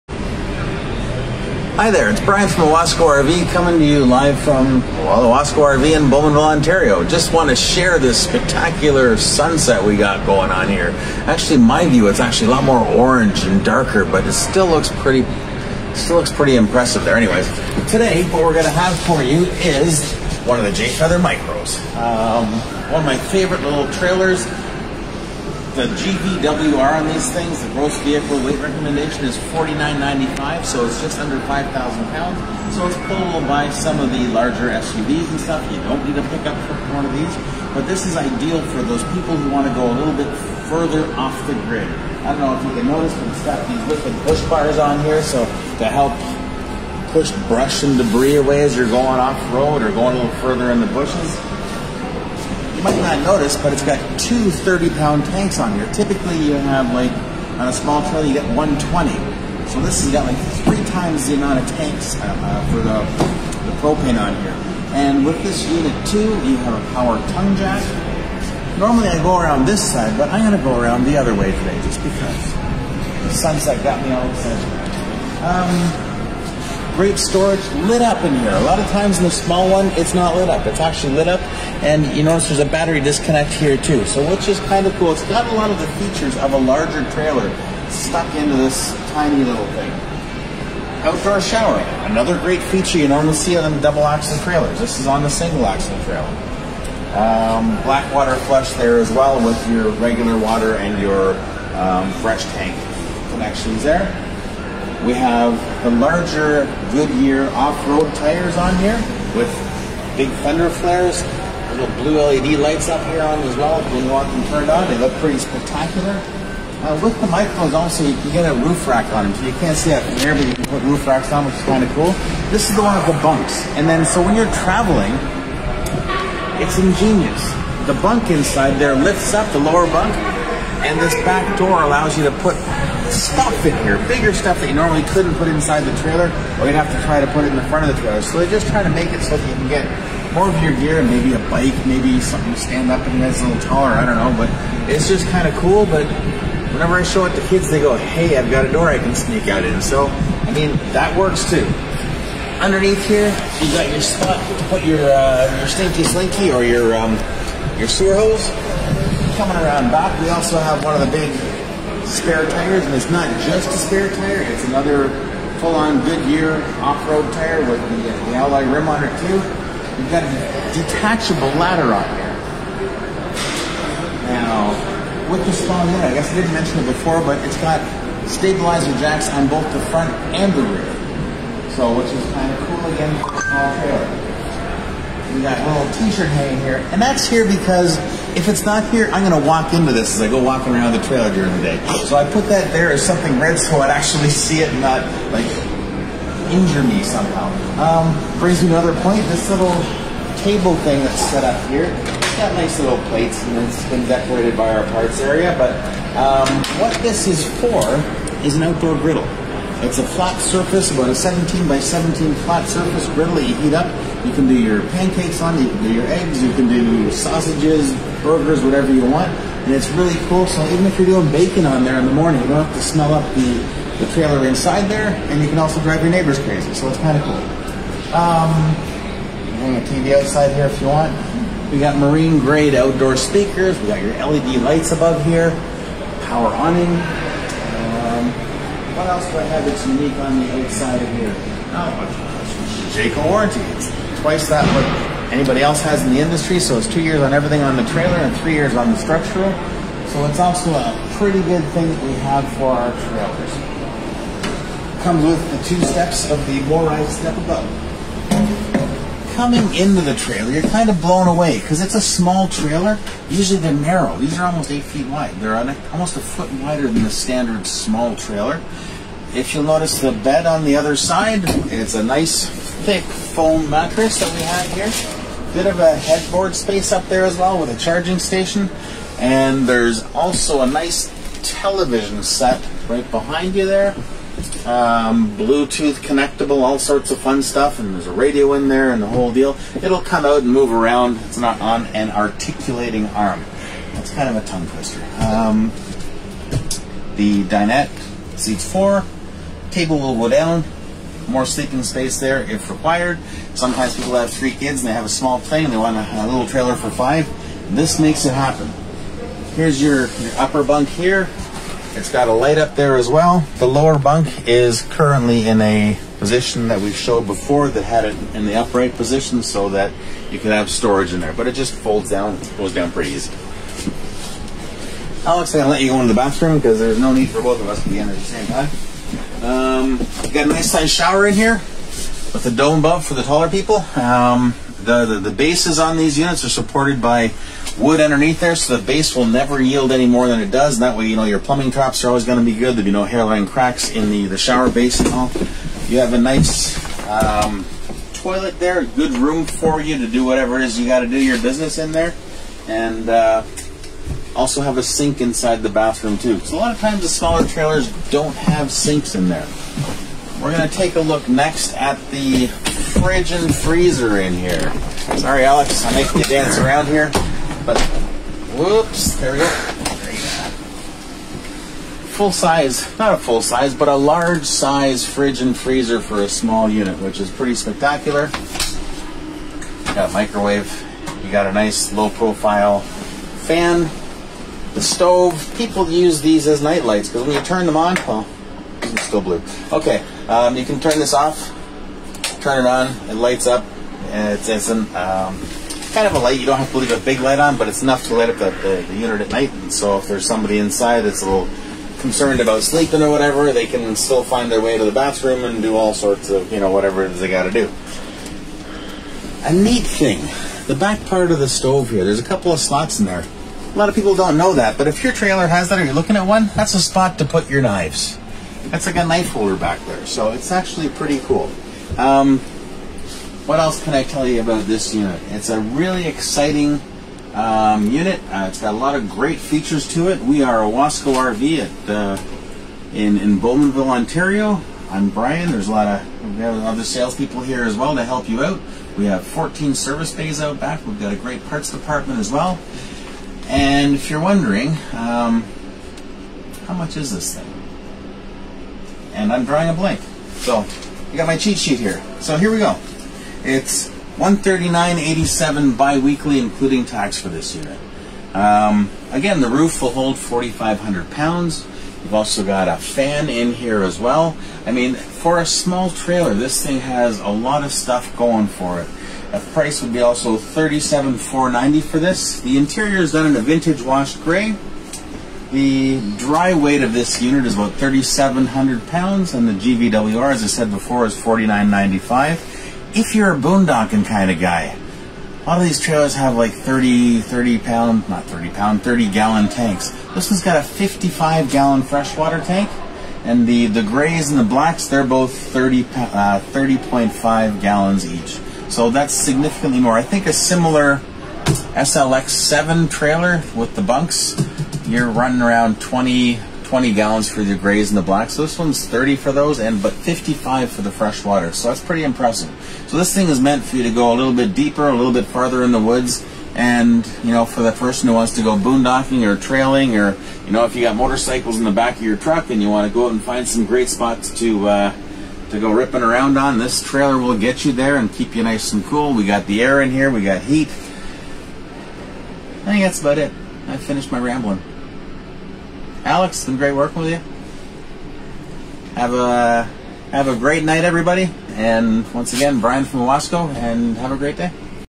2022 Jayco Jay Feather Micro 171BH | Facebook Live @ 5 | Owasco RV Centre
Join Us On Facebook Every Thursday At 5pm For Owasco RV Product Presentations 🏞️